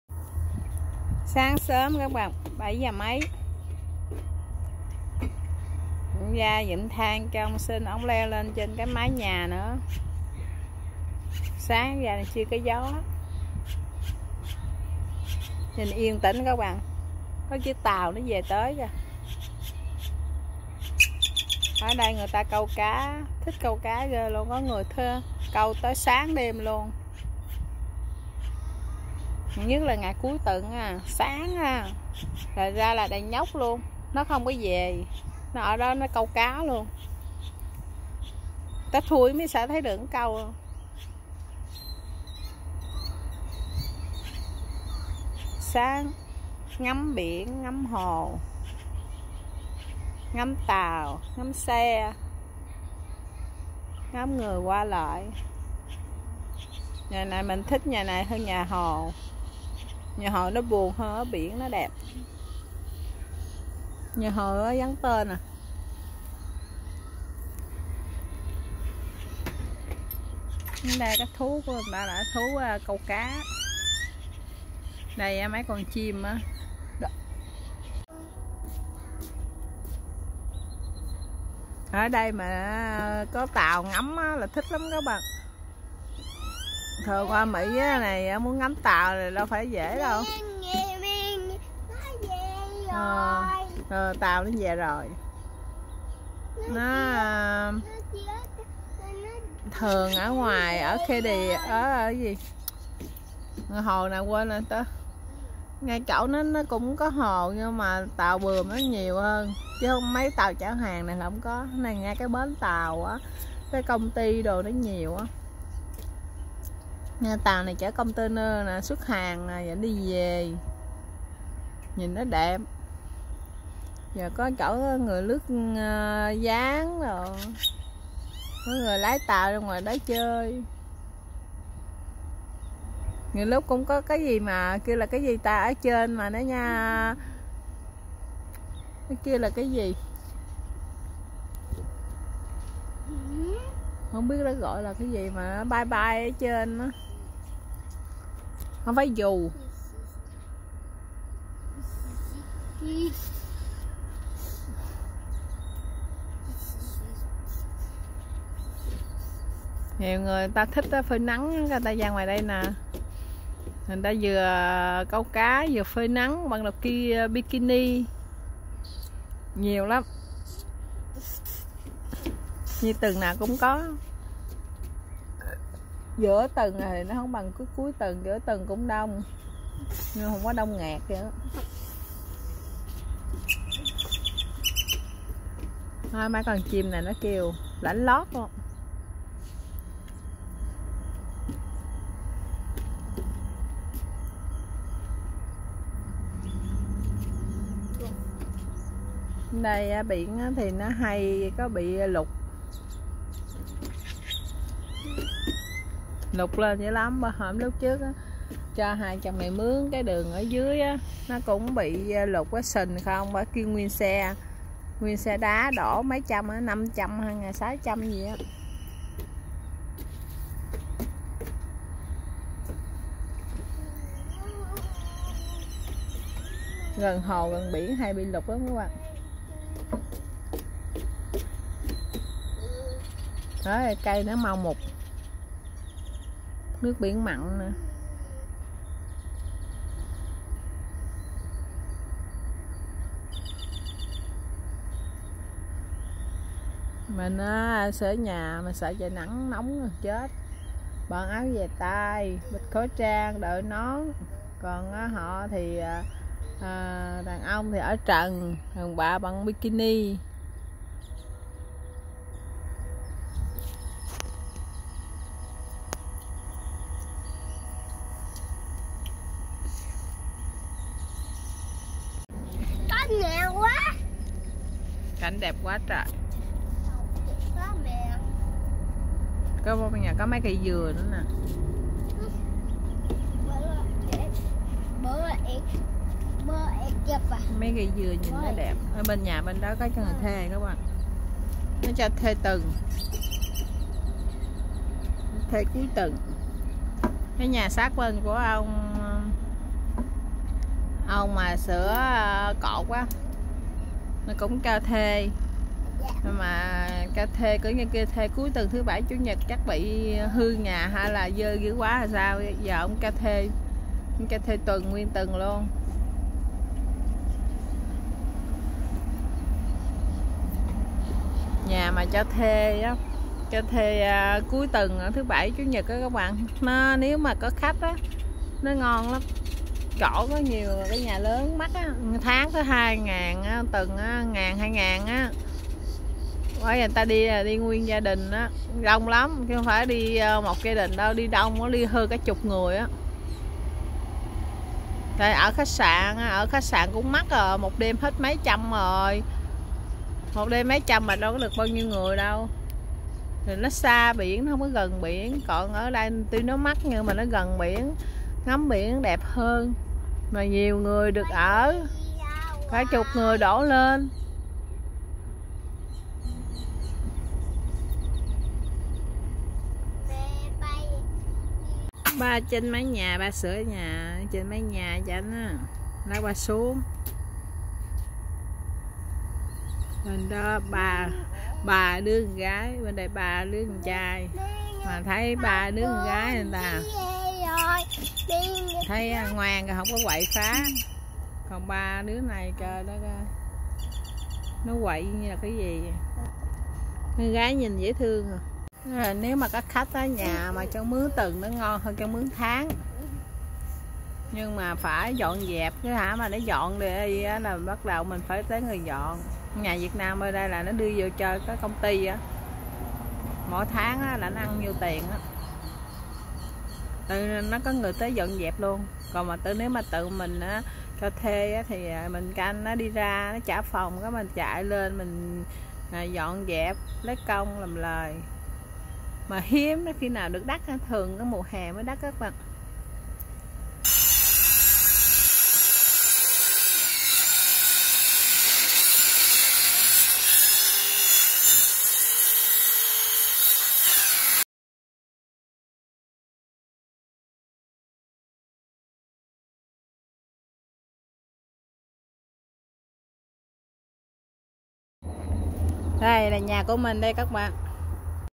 Có ai thích biển kg nè, cùng hứng gió và ngắm biển và tàu với Ngọc nè.
Có ai thích biển kg nè, cùng hứng gió biển và ngắm biển và tàu với Ngọc  nè